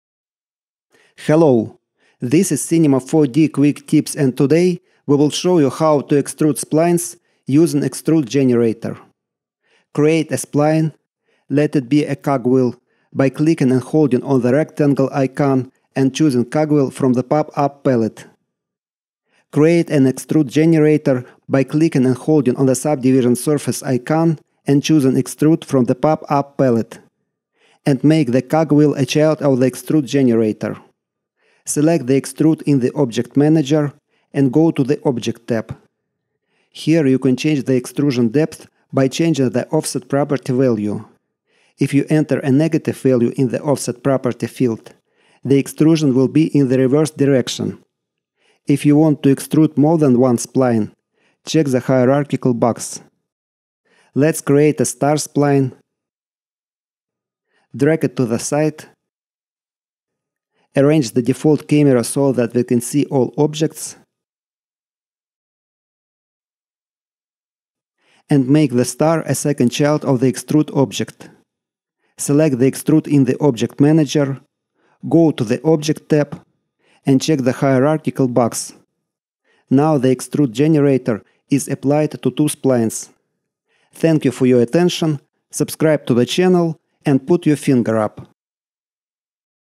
Cinema 4D: How to extrude splines
How to extrude splines using Extrude generator in Cinema 4D.